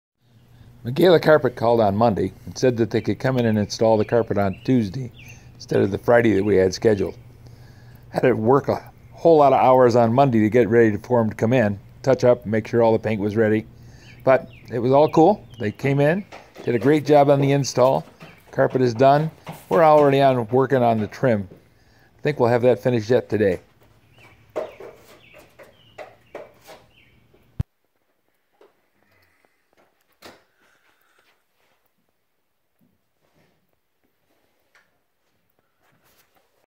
Flood Damage - Carpet Replaced
Migala Carpet One called and said that they could come Tuesday and put the carpet in instead of waiting for Friday's appointment.